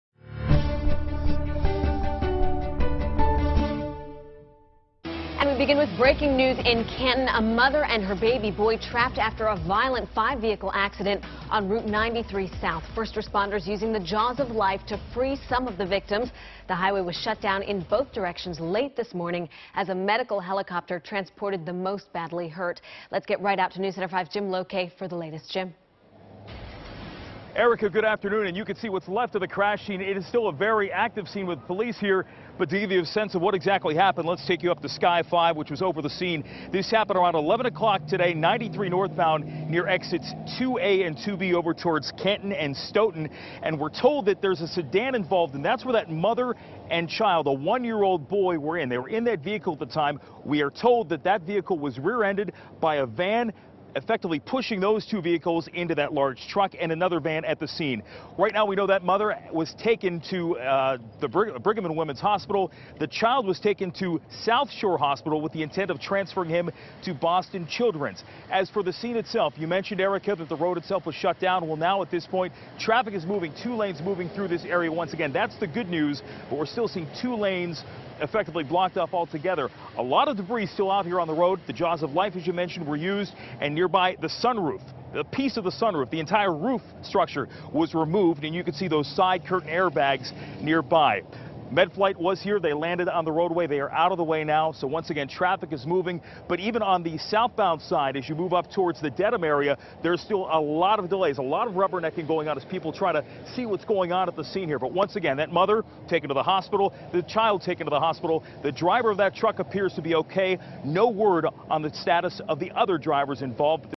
Mom, baby seriously injured in crash
A mother and her 1-year-old son were seriously injured in a crash in Canton.